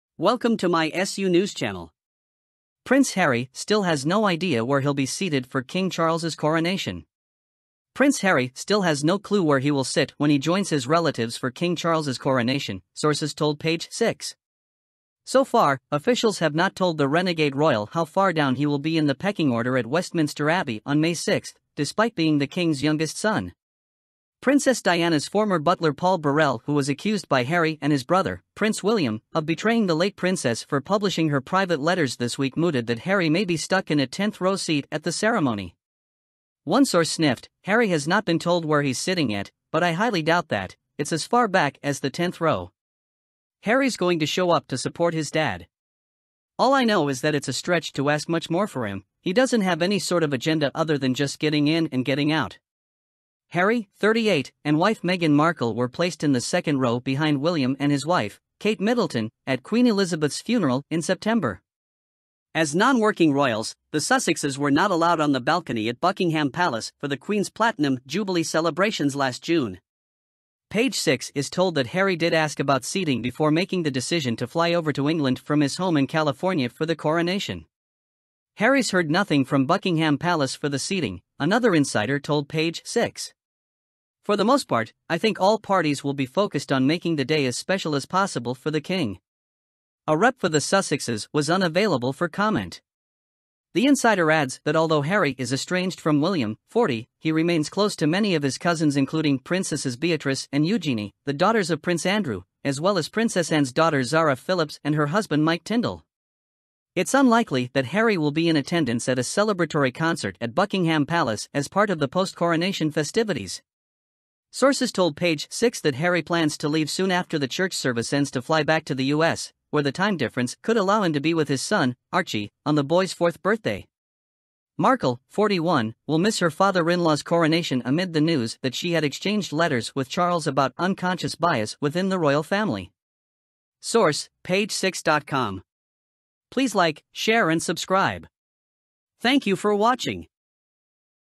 News: Prince Harry still has no idea where he’ll be seated for King Charles’ coronation, SUNews
1. Video Quality
2. Type of Breaking News and News Clip Content that you want.